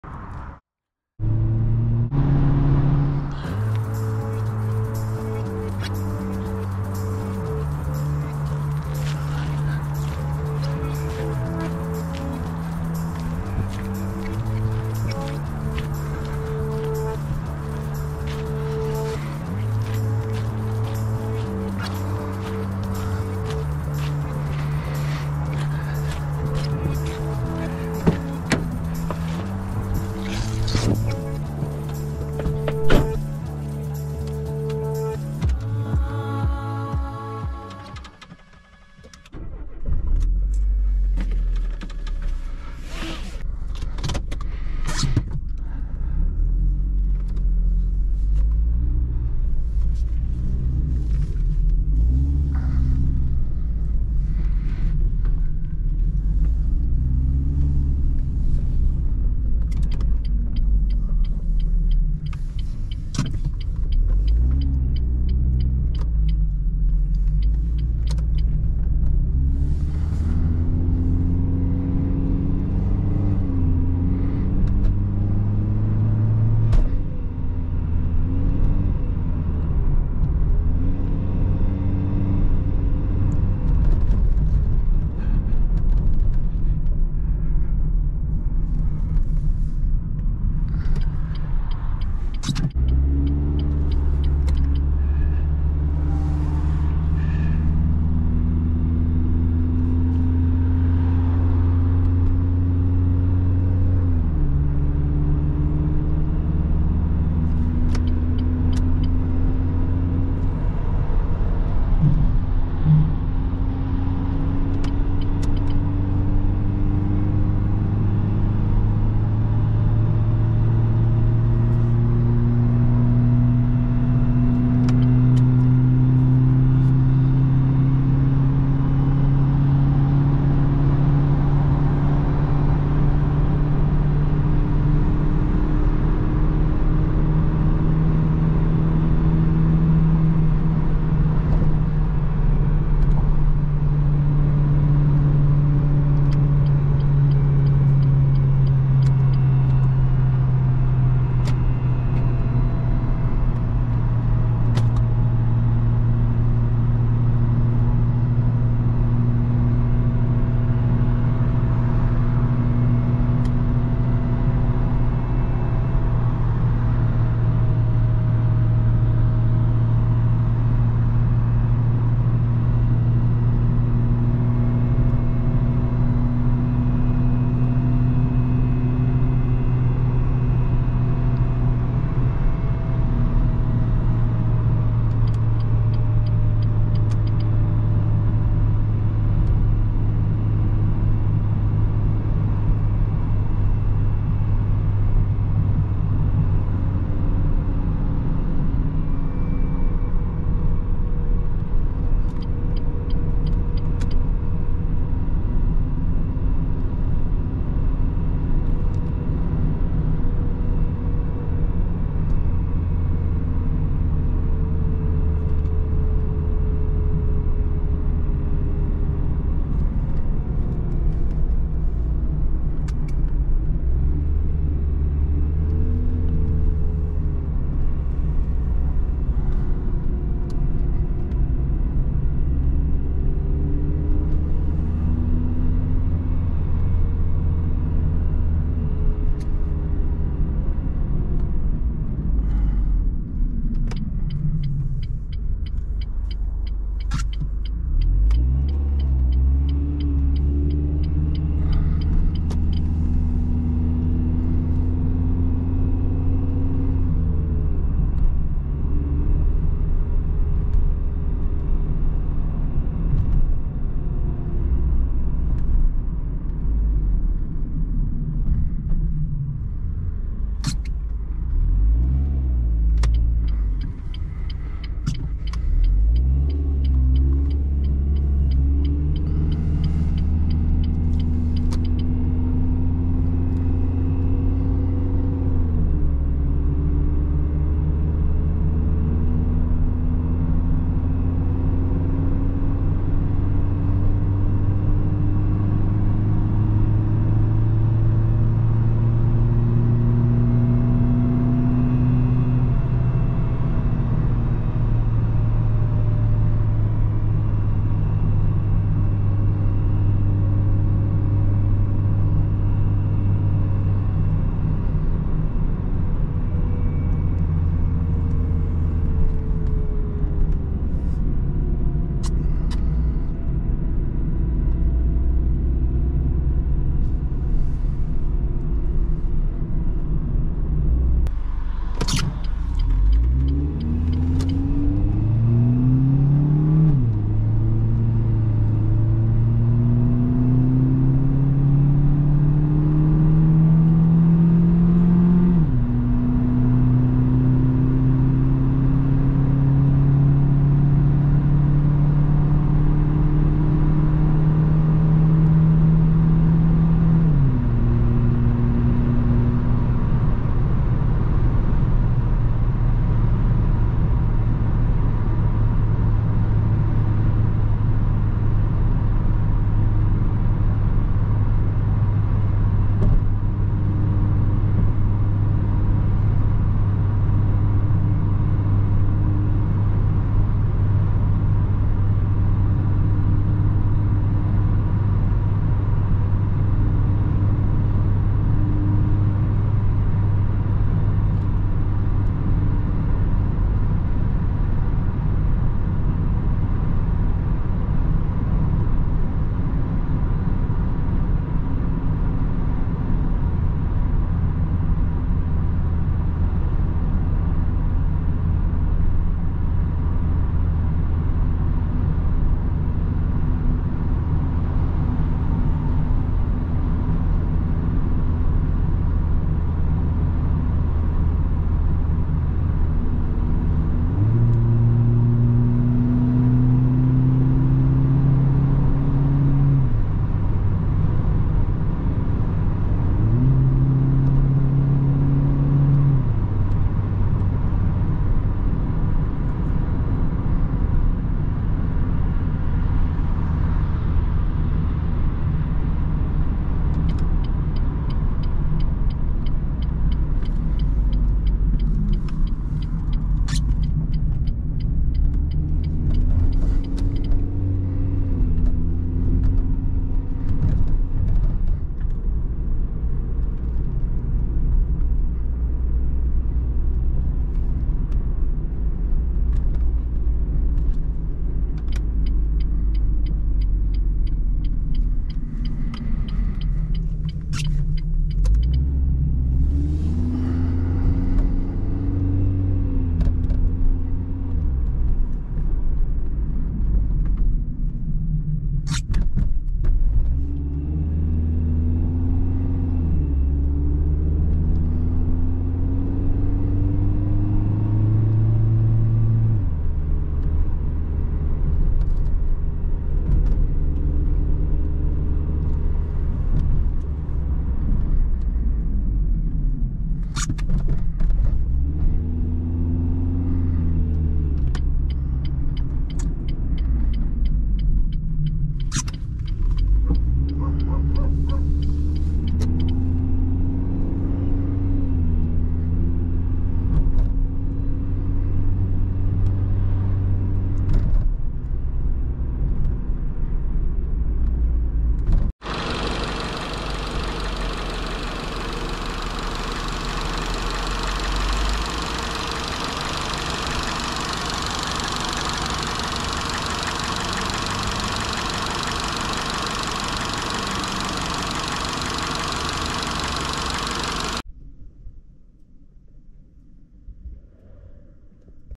2005 Renault Clio [1.6 I 16V 110HP] |0-100| POV Test Drive #1607 Joe Black
Car: Renault Clio III Hatchback 5d 1.6 I 16V 110HP 2005

0:00 Intro
0:09 Walk-around and interior
0:45 Casual driving
1:55 Highway
3:10 Casual driving
5:31 Acceleration
5:53 Casual driving
8:56 Engine view
9:15 Fuel Consumption

Engine: 1.6L Petrol (Gasolina) 4-cylinders
Displacement (cc): 1598
Power: 81 kW/110 hp
Torque: 150 Nm
Transmission: 4-speed automatic
Traction: FWD ( Front-Wheel Drive )

Acceleration (0–100 km/h): 12,2 s.
Fuel Consumption Combined in l/100 km by Renault: 7,5

Fuel Tank Capacity: 55L
Total Range: 733 km
Weight: 1265 Kg
Cargo Space: 288 Liter
Model: 2005
Date of Production : 2005 – 2012
Base Price in Germany : 18.250 EUR

Driver: Joe Black
Video: GoPro Hero 11 Black / Sony a6300 + Sigma 16mm f1.4
Audio: Zoom H2n (recorder) & Roland CS-10EM (binaural microphones)
Edit: MacBook Pro / Final cut Pro X